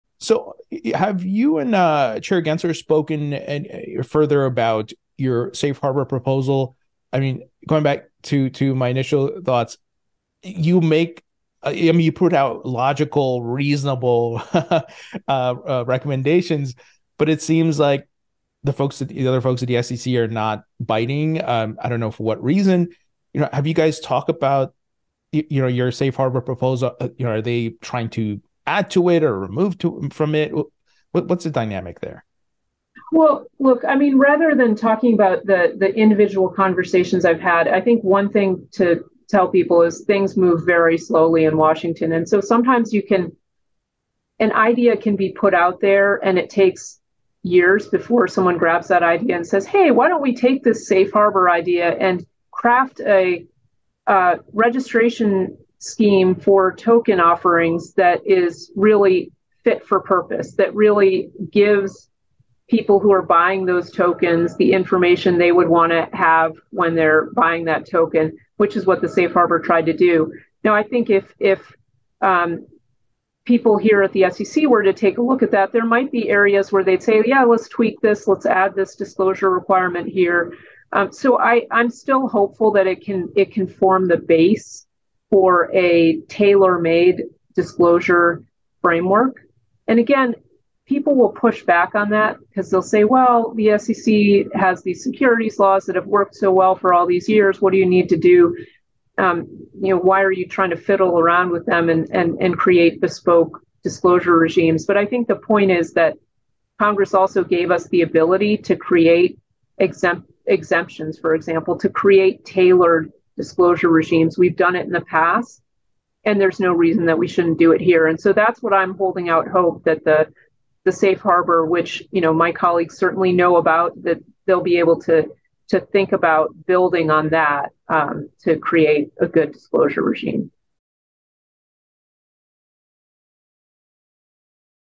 Has Gary Gensler Discussed Hester Peirce's Safe Harbor Crypto Proposal?
SEC Commissioner Hester Perice talks if Chair Gary Gensler discussed her safe harbor proposal
🔥Sign up with OKX, one of the world's largest crypto platforms

⏰ Time Stamps ⏰
00:00 Intro